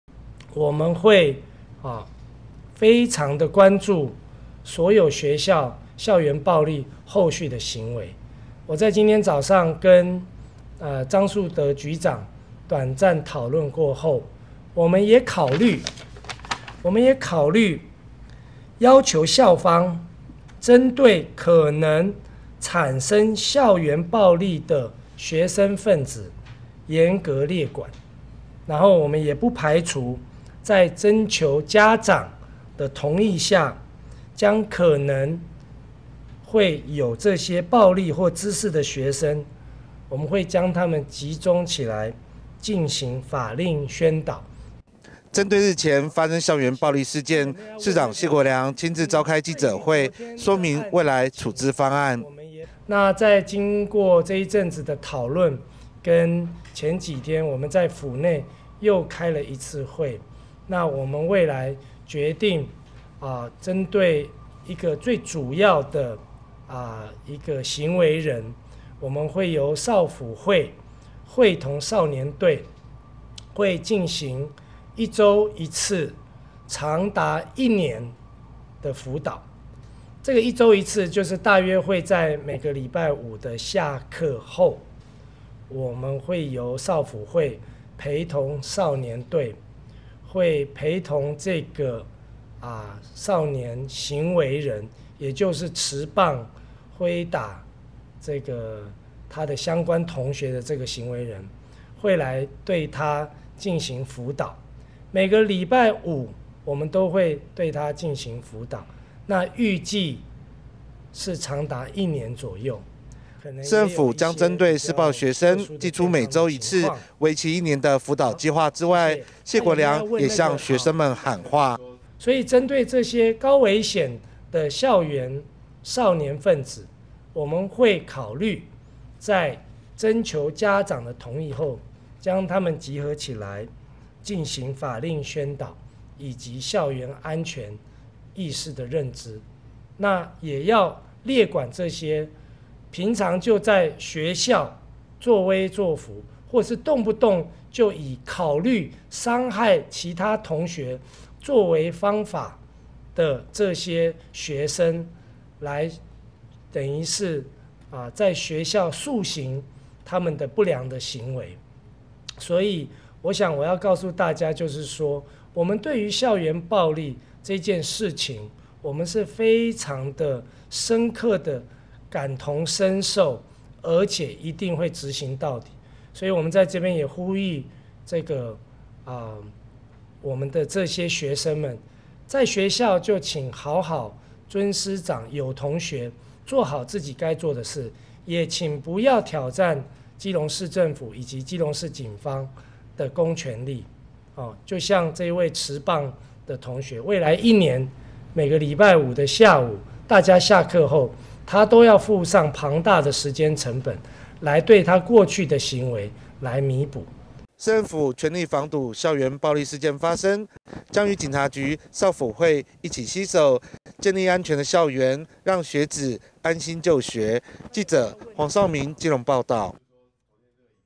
校園暴力 ｜謝國樑｜遏制校園暴力 謝國樑祭一年期輔導計畫
(中嘉吉隆新聞 記者黃紹銘 /基隆報導) 基隆市日前發生學生持球棒毆打同學的校園暴力事件，基隆市市長謝國樑經過與校方、警方及輔導人員進行會議後，決定將針對傷人的同學啟動長達一年期的輔導計畫，在市府少輔會、警察局少年隊的陪同下，將每周對施暴同學進行輔導。
針對日前發生校園暴力事件，市長謝國樑親自召開記者會，說明未來處置方式。
市政府將針對施暴的行為人，祭出每周一次，為期一年輔導計畫之外，謝國樑也向行為偏差的學生們喊話。
市府全力防堵校園暴力事件發生，將與警察局、少輔會一起攜手，建立安全校園，讓學子安心就學。